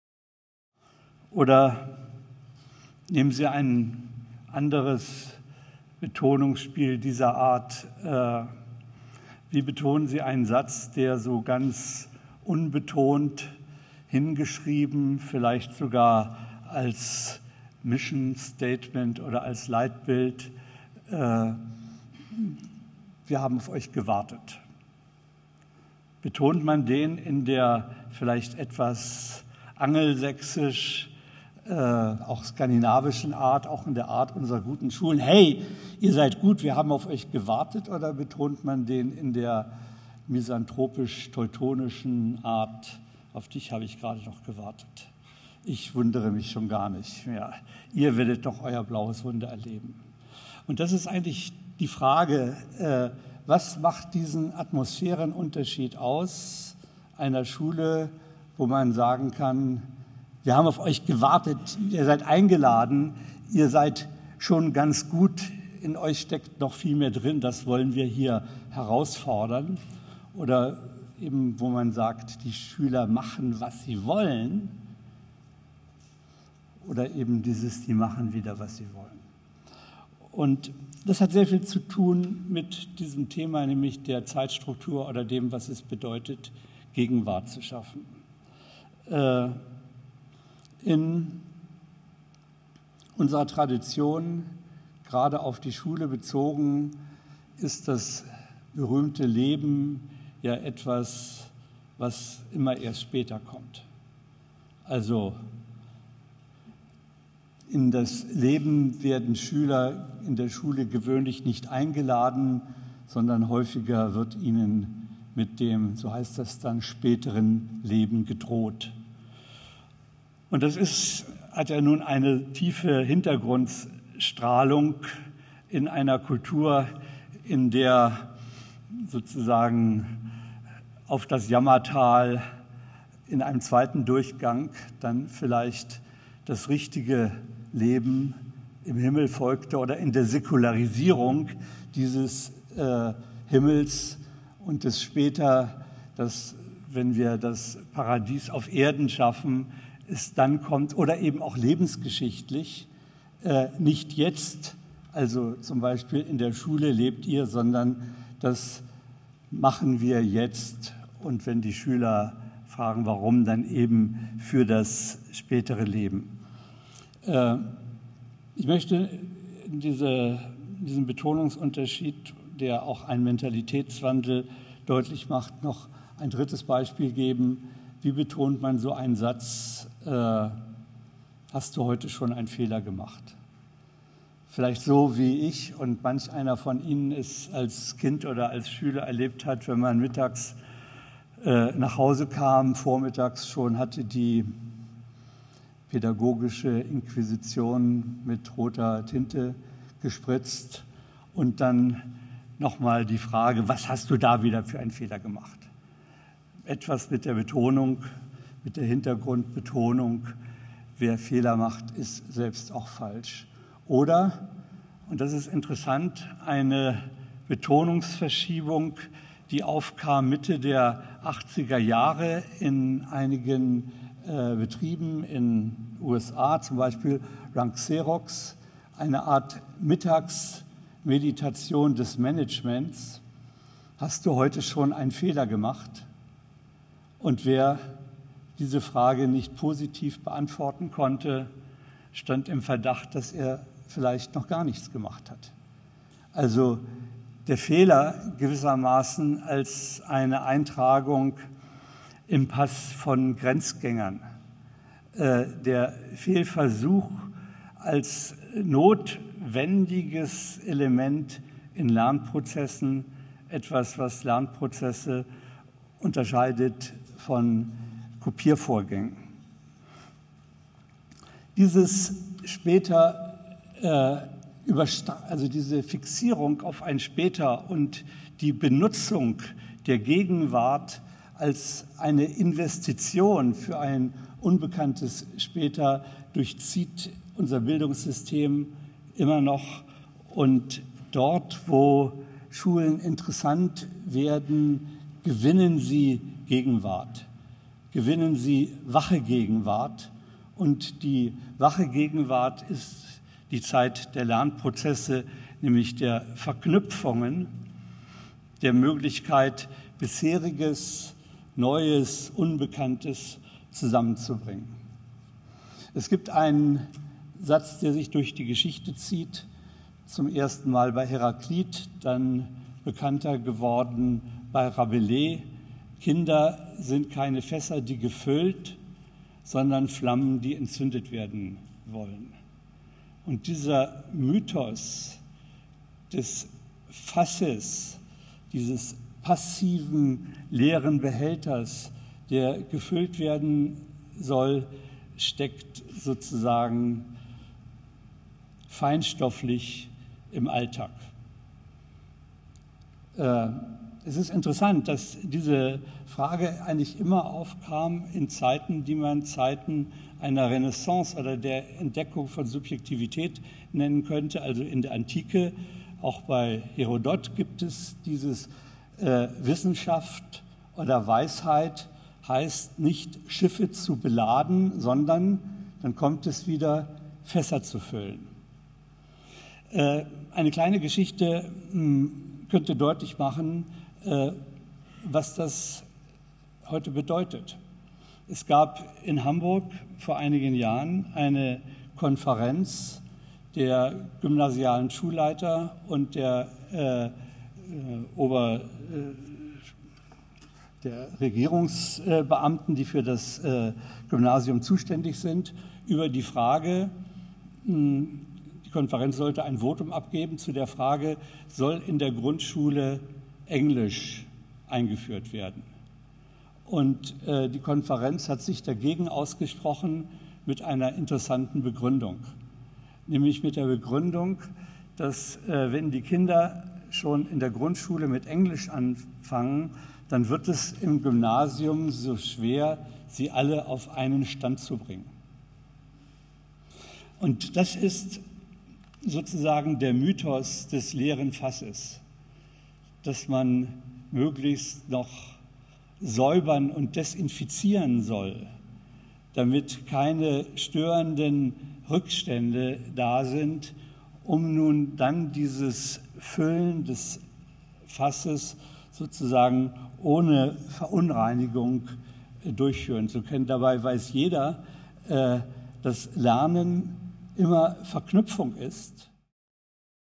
Reinhard Kahl: Gegenwart! Vorschläge für die Schule der Zukunft (2/4)
Aufzeichnung der Mosse-Lecture vom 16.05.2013 in der Humboldt-Universität zu Berlin.